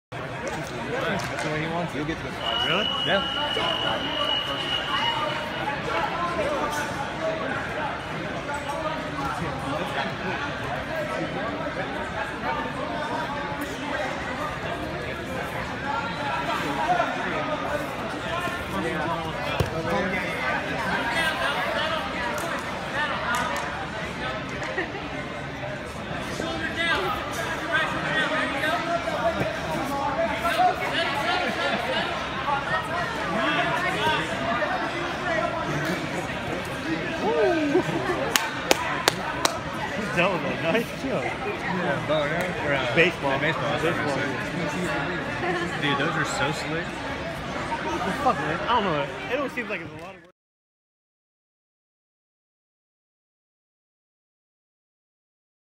Baseball Bat Choke (Championship)
Baseball Bat Choke in the finals of gi at the 2017 AGF Kansas City Championships.